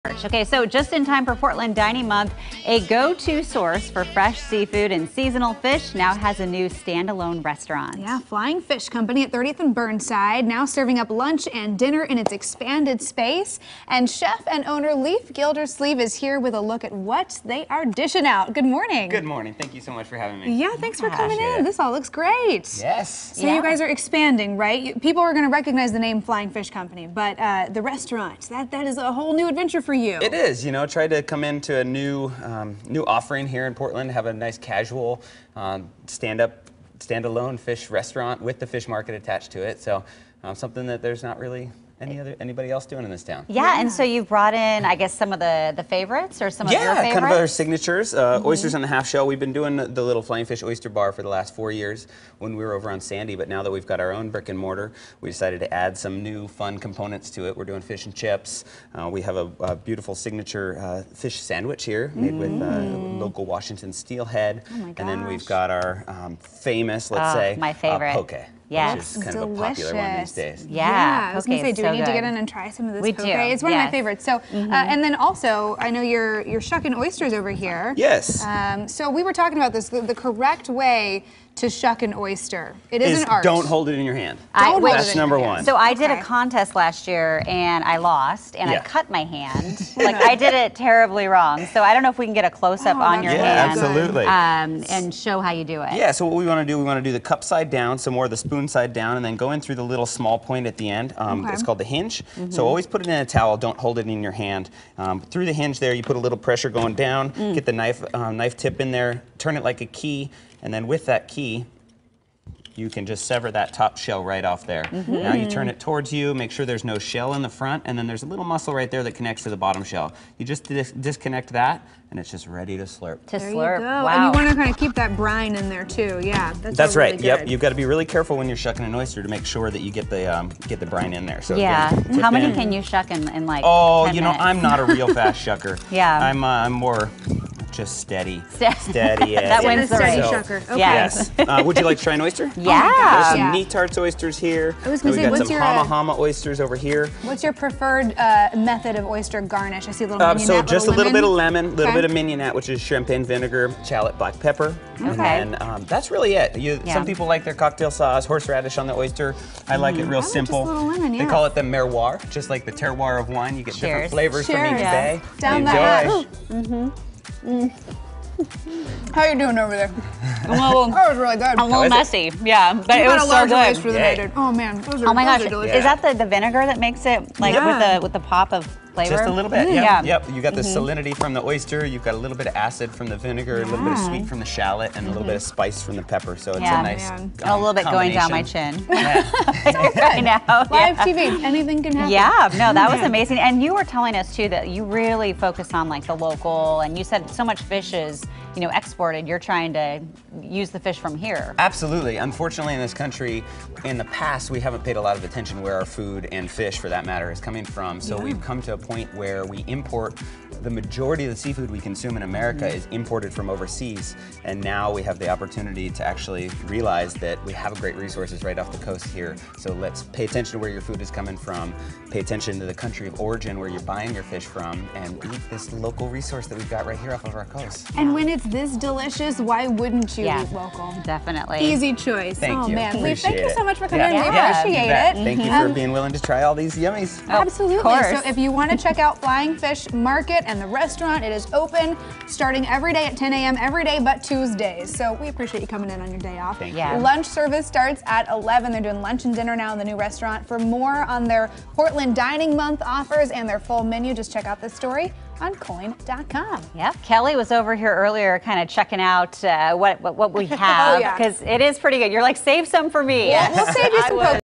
Portland's Flying Fish jumps from cart to restaurant
Flying Fish just opened its first standalone seafood restaurant, oyster bar and fish market at 30th and East Burnside.